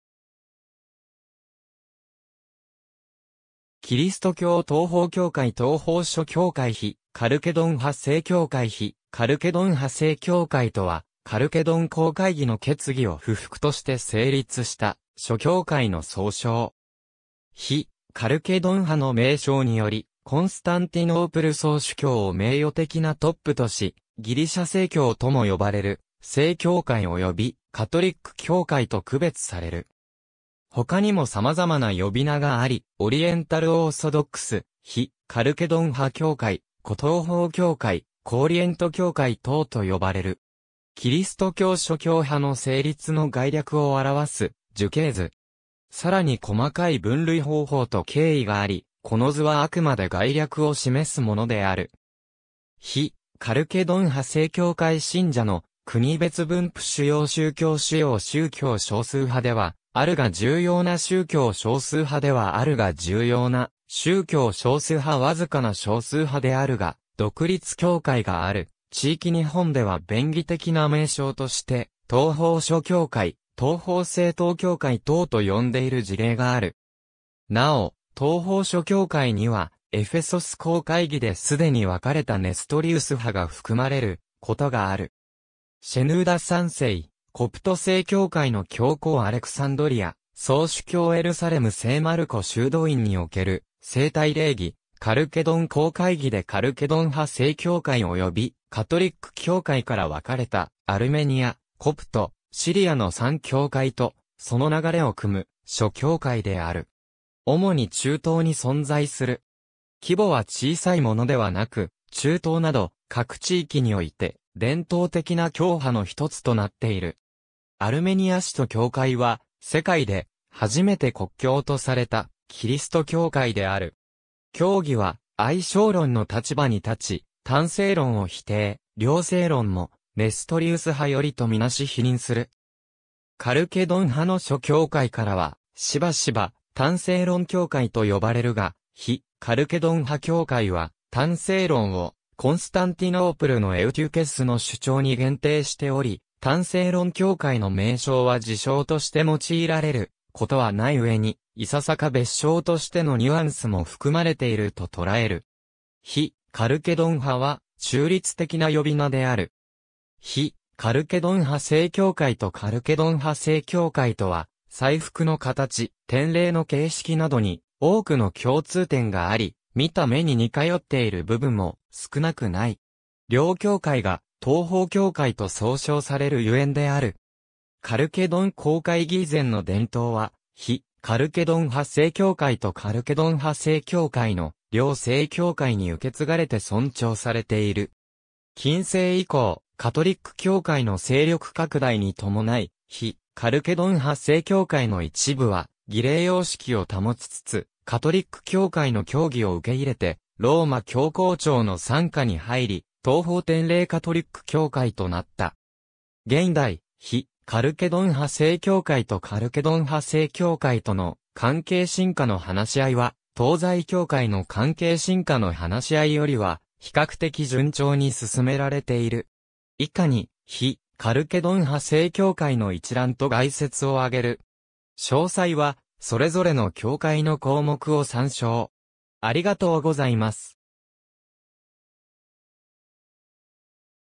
非カルケドン派正教会
キリスト教 」 東方教会 」 東方諸教会 」 非カルケドン派正教会 非カルケドン派正教会（ひカルケドンはせいきょうかい、英語: Non-Chalcedonian Orthodox Churches）とは、カルケドン公会議（第四全地公会議）の決議を不服として成立した諸教会（アルメニア使徒教会、コプト正教会、シリア正教会など）の総称。
「非カルケドン派」の名称により、コンスタンティノープル総主教を名誉的なトップとし「ギリシャ正教」とも呼ばれる正教会及びカトリック教会（カルケドン派）と区別される。
他にも様々な呼び名があり、オリエンタル・オーソドックス（オリエント正教、英語: Oriental Orthodox）、非カルケドン派教会（ロシア語: Дохалкидонские церкви）、古東方教会（ロシア語: Древневосточные Церкви）、古オリエント教会（イタリア語: Chiese orientali antiche, ドイツ語: Altorientalische Kirchen）等と呼ばれる。
キリスト教諸教派の成立の概略を表す樹形図。
更に細かい分類方法と経緯があり、この図はあくまで概略を示すものである。
非カルケドン派正教会信者の国別分布 主要宗教 (75%以上) 主要宗教 (50～75%) 少数派ではあるが重要な宗教 (20～50%) 少数派ではあるが重要な宗教 (5～20%) 少数派 (1～5%) 僅かな少数派であるが (1%未満)、独立教会がある地域 日本では便宜的な名称として東方諸教会、東方正統教会等と呼んでいる事例がある。
なお、「東方諸教会」には、エフェソス公会議で既に分れた（つまり「非エフェソス派教会」であって「非カルケドン派教会」ではない）ネストリウス派（アッシリア東方教会など）が含まれることがある（この場合、非カルケドン派＋ネストリウス派＝東方諸教会）。
シェヌーダ3世：コプト正教会の教皇アレクサンドリア総主教 エルサレム聖マルコ修道院における聖体礼儀（シリア正教会） カルケドン公会議でカルケドン派正教会およびカトリック教会から分かれたアルメニア、コプト（エジプト）、シリアの3教会と、その流れを汲む諸教会である。
主に中東に存在する。
規模は小さいものではなく、中東など各地域において伝統的な教派の一つとなっている。
アルメニア使徒教会は世界で初めて国教とされたキリスト教会である。
教義は合性論の立場に立ち、単性論を否定、両性論もネストリウス派寄りと見なし否認する。
カルケドン派の諸教会からは、しばしば「単性論教会」と呼ばれるが、非カルケドン派教会は単性論をコンスタンティノープルのエウテュケスの主張に限定しており、「単性論教会」の名称は自称として用いられることはない上に、いささか蔑称としてのニュアンスも含まれていると捉える。
非カルケドン派は中立的な呼び名である。
非カルケドン派正教会とカルケドン派正教会とは、祭服の形・典礼（奉神礼）の形式などに多くの共通点があり、見た目に似通っている部分も少なくない。
両教会が東方教会と総称される所以である。
カルケドン公会議以前の伝統は非カルケドン派正教会とカルケドン派正教会の両正教会に受け継がれて尊重されている。
近世以降、カトリック教会の勢力拡大に伴い、非カルケドン派正教会の一部は儀礼様式を保ちつつカトリック教会の教義を受け入れてローマ教皇庁の傘下に入り、東方典礼カトリック教会となった。
現代、非カルケドン派正教会とカルケドン派正教会との関係深化の話し合いは、東西教会の関係深化の話し合いよりは比較的順調に進められている。
以下に非カルケドン派正教会の一覧と概説を挙げる。
詳細はそれぞれの教会の項目を参照。